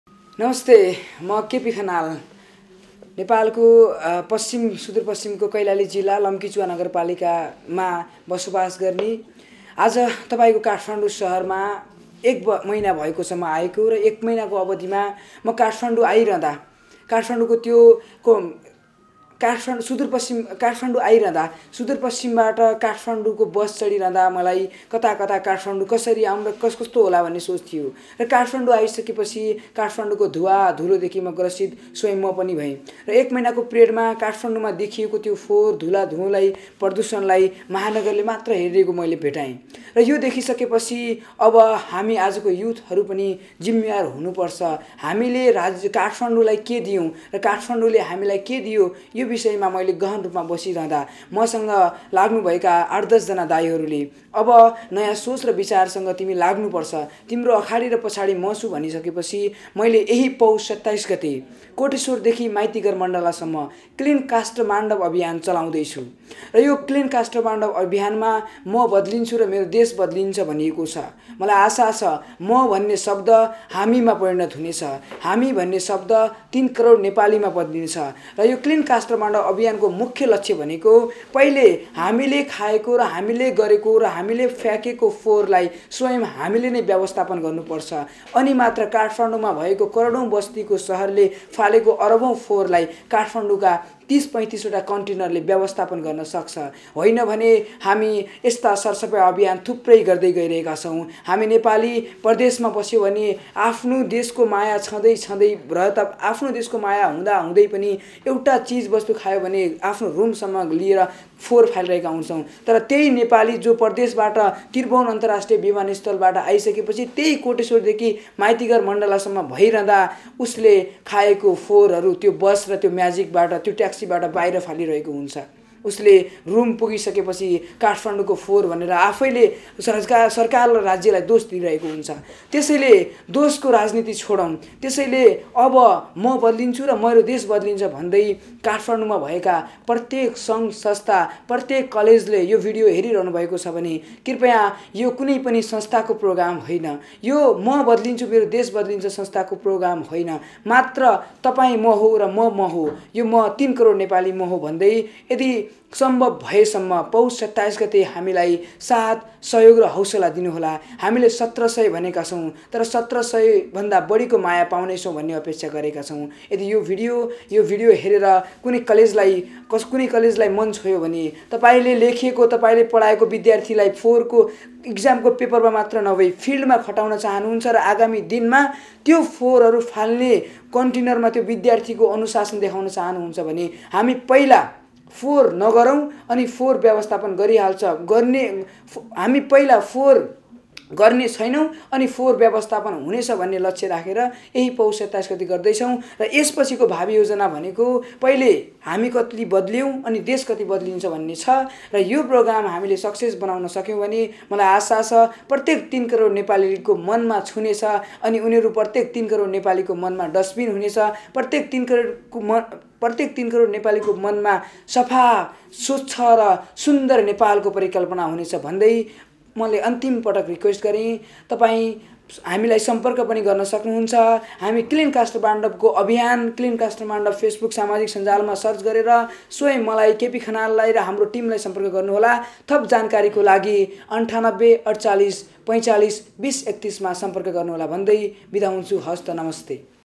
क्लीन काष्ठमाण्डप म बदलिन्छृ र देश बदलिन्छ -NEWS 24
आम सर्बसाधरणहरुमा फोहोरमैला सम्बन्धी जनचेतना जगाउन क्लीन काष्ठमाण्डप म  बदलिन्छृ र देश बदलिन्छ अभियान शुरु हुने भएको छ । १७ वर्षीय केपी खनालको संयोजकत्वमा युवाहरुको टोलीले पुष २७ गते कोटेश्वर देखि माइतीघरसम्म सडकमा रहेका प्लाष्टिजन्य लगायत फोहोरहरुको व्यवस्थापन गर्ने बताएका छन् । राजधानीमा फोहोरमैलाको व्यवस्थापन हुन नसक्दा पछिल्लो समय प्रदुषित हुँदै गएको भन्दै युवाहरुको सक्रियतामा क्लीन काष्ठमाण्डप अभियान शुरु हुन लागेको हो ।